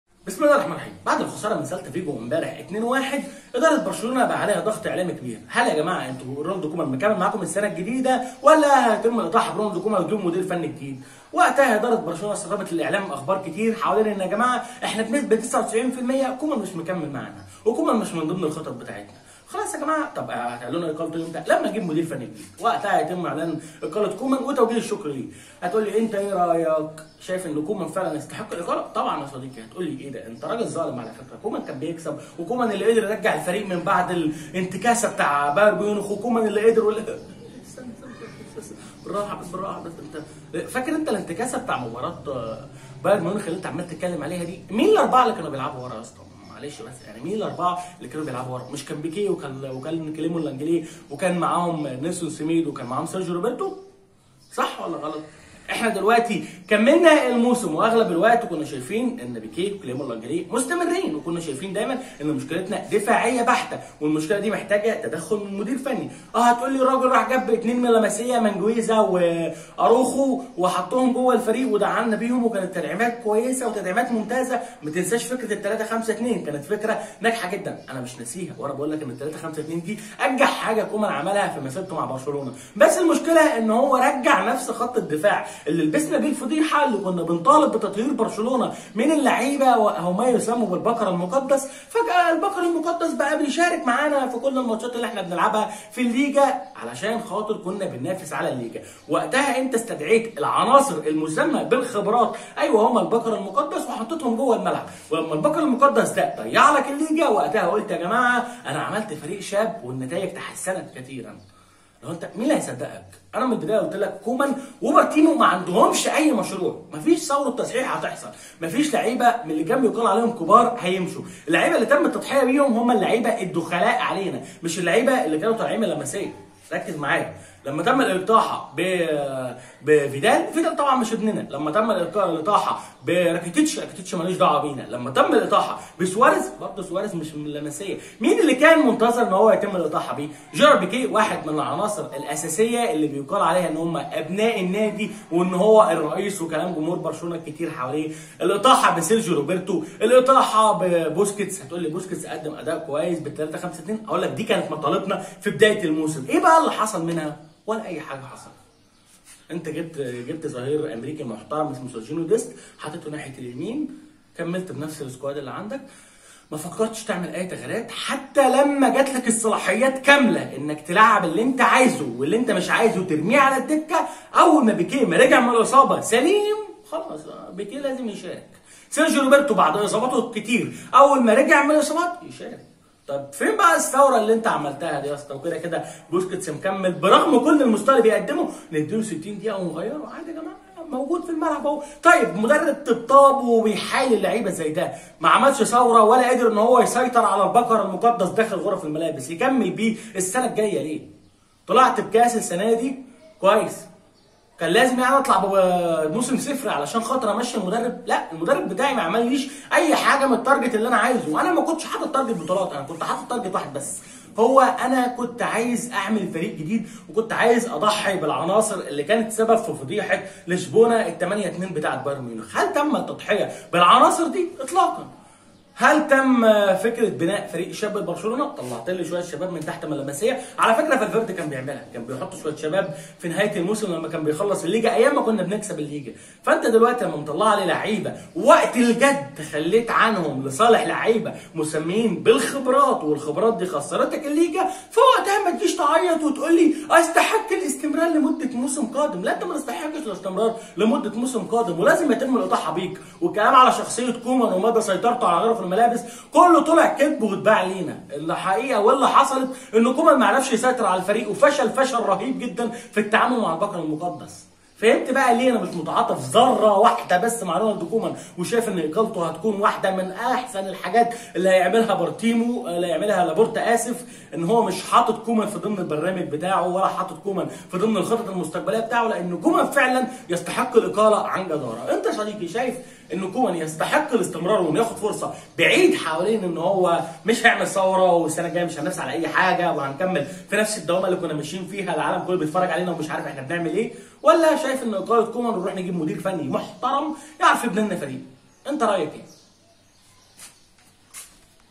برشلونه يقرر اقاله رونالد كومان بعد خساره الدوري وعوده الابقار المقدسه
بعد خساره برشلونه لقب الليجا رسميا بحث الجميع عن شئ واحد وهو هل يستمر رونالد_كومان مع برشلونه او لا لياتي الرد سريعا بان اداره برشلونه قررت#اقاله_رونالد_كومان من ادريب الفريق